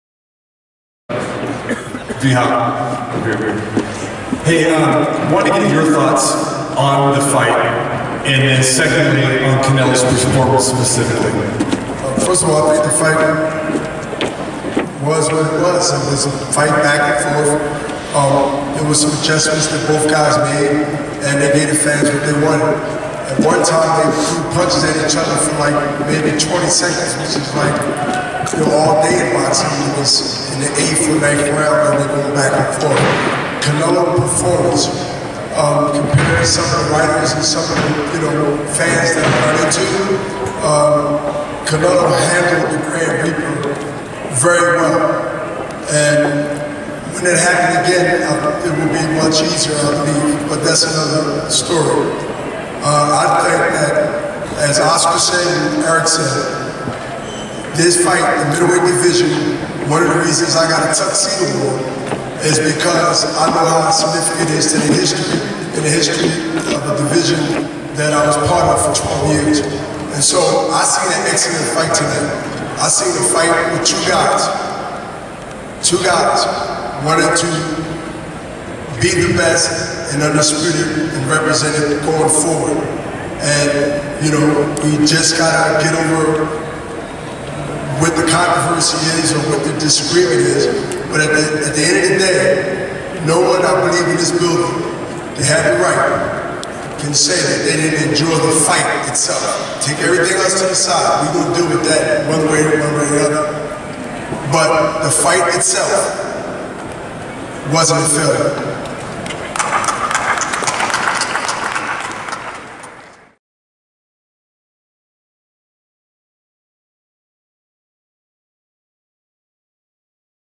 BERNARD HOPKINS GIVES PASSIONATE SPEECH ABOUT DRAW IN CANELO VS. GOLOVKIN; PRAISES FIGHTERS
FightHype.com was on hand at the T-Mobile Arena in Las Vegas, Nevada where undefeated unified middleweight champion Gennady Golovkin and lineal champion Canelo Alvarez fought to a draw after 12 hard rounds. Check out the scene immediately after the fight as both fighters spoke to the media in attendance.